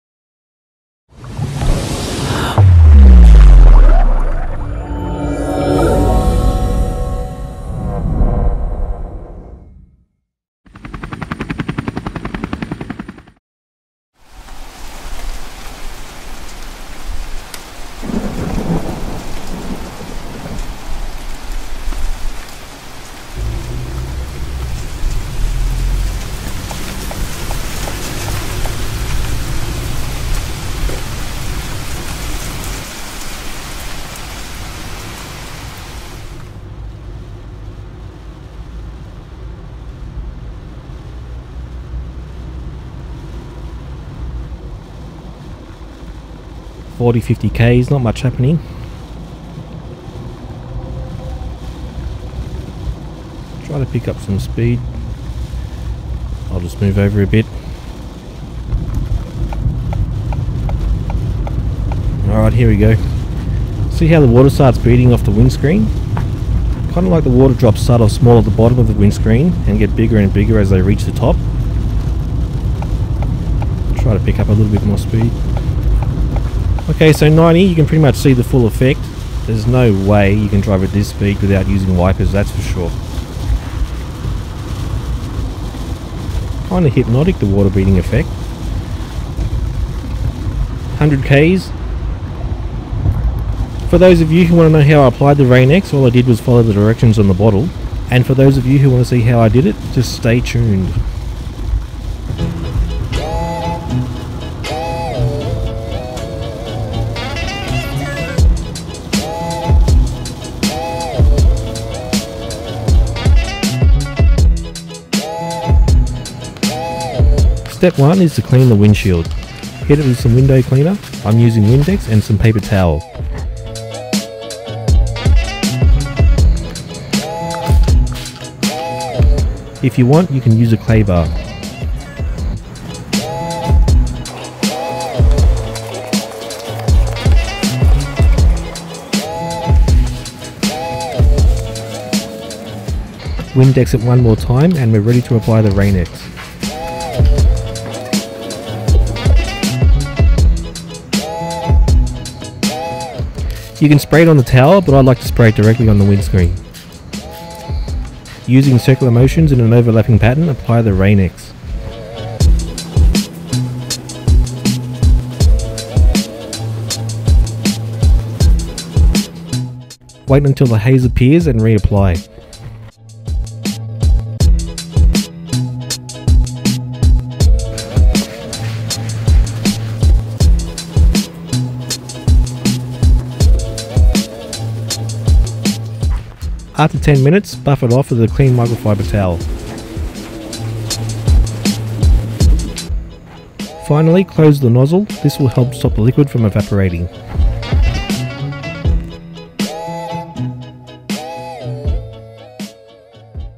RAIN-X Water Repellent. 100km/h Driving Test
Actual driving test after applying Rain-x to my windshield.
Test and review of rain x water repellent.
100km per hour highway test.